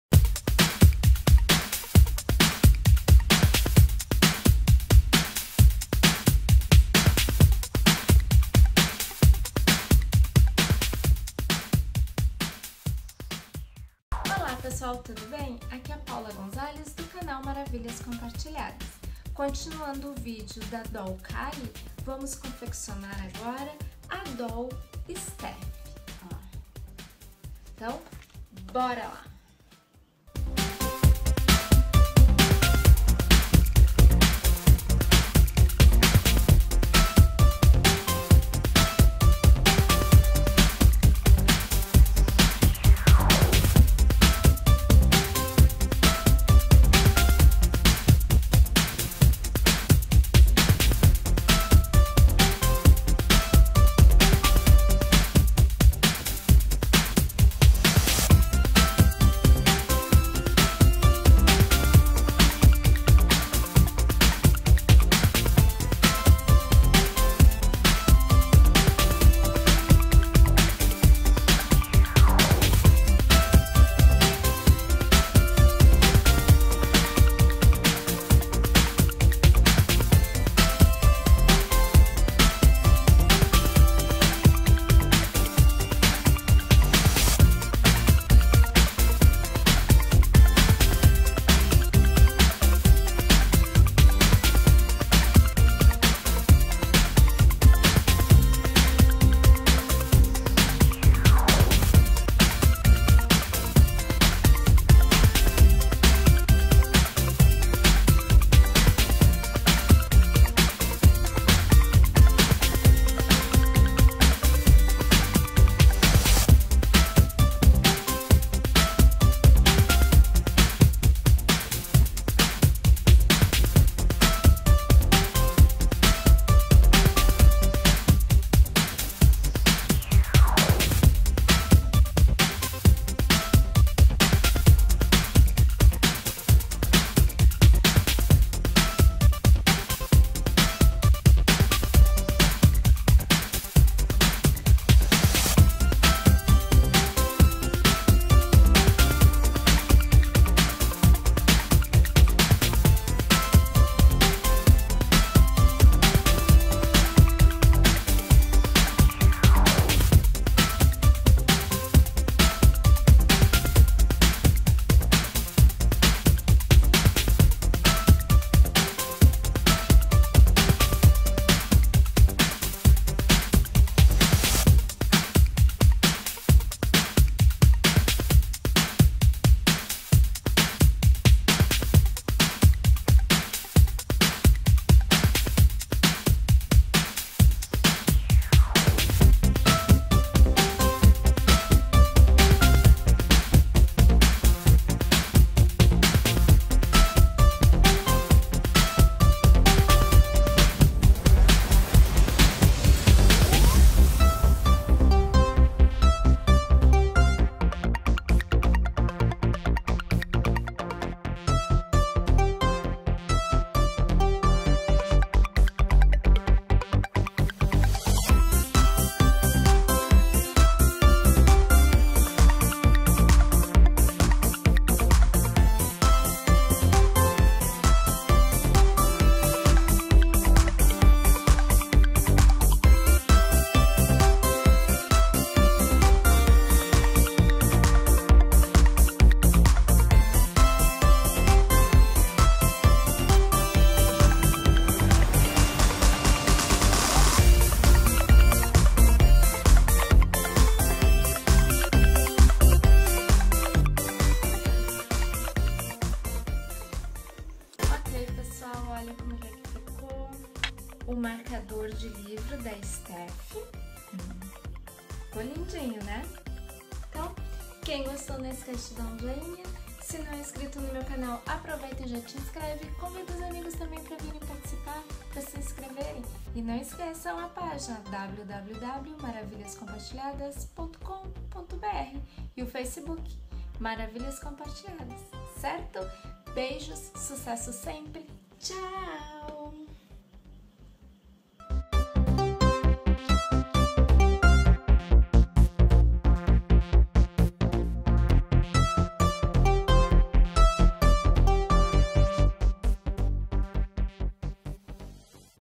Marcador de Livros da Doll Steffi - Maravilhas Compartilhadas
Olá Pessoal, continuando o vídeo do marcador de livros das Dolls,  vamos confeccionar agora o da Doll Steffi... boraaa...

Os materiais utilizados, comprei na Loja Cibideias da Cibele Degrazia que fica em Tatuapé/São Paulo, Loucas por caixas (carimbos) e outros em lojinhas aqui da minha cidade e compras pela internet.

Também utilizei materiais que ganhei do Sérgio Vasconcelos O Cartoneiro e da Ana Paula, das troquinhas de Páscoa e sorteios na Loja Cibideias.

Material: Carimbo Doll Steffi, Carimbeira, papel canson,  papel de Scrapbook, tesoura, cola de silicone, estilete, lápis de cor nas cores: preto, bege cor da pele, azul, laranja, dourado, cola glitter nas cores: fruta cor, vermelho, furador crop a day, retalho de juta,  renda de papel, cordão rosa, fita banana...

Facebook Maravilhas Compartilhadas
Istagram @maravilhascompartilhadas
Google+ +Maravilhas Compartilhadas
Snapchat maravilhascompa
Periscope @maravilhasComparti

Bjs sucesso sempre!
Maravilhas Compartilhadas By Paula Gonzales